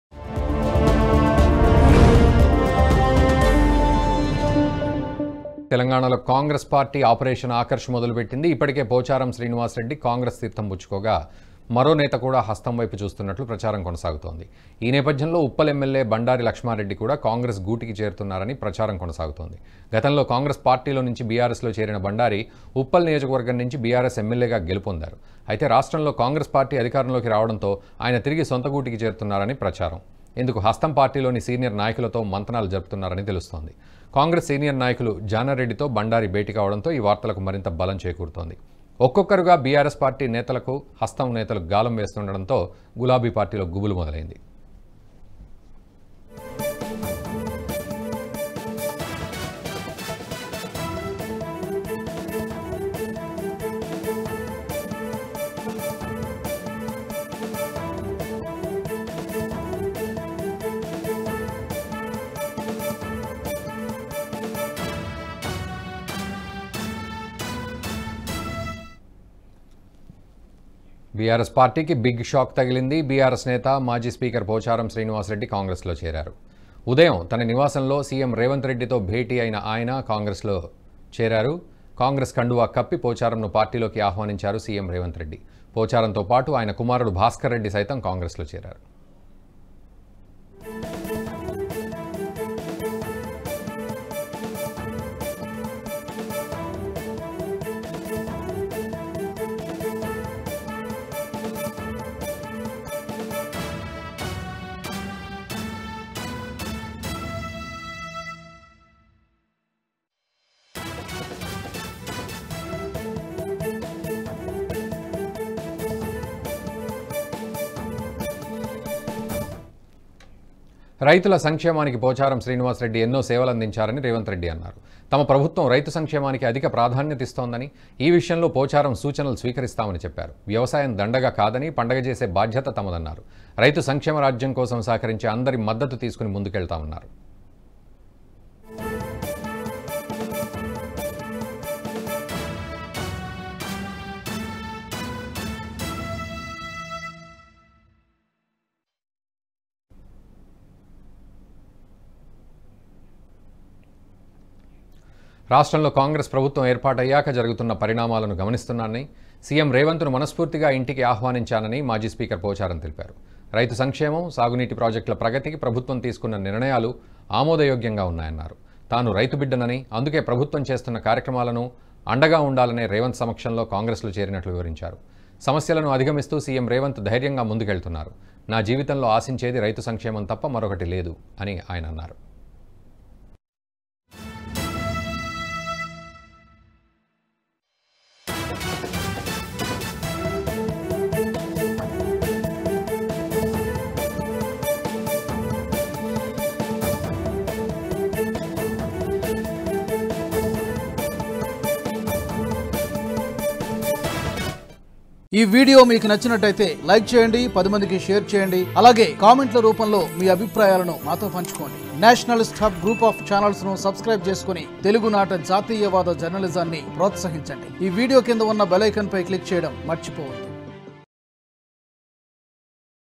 BRS Pocharam Srinivas Reddy Joins Congress | CM Revanth Reddy | Telangana News | NHTV
Our Company account details :
Name : DHARMAGNYA BROADCASTING PVT LTD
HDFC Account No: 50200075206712
IFSC Code: HDFC0008045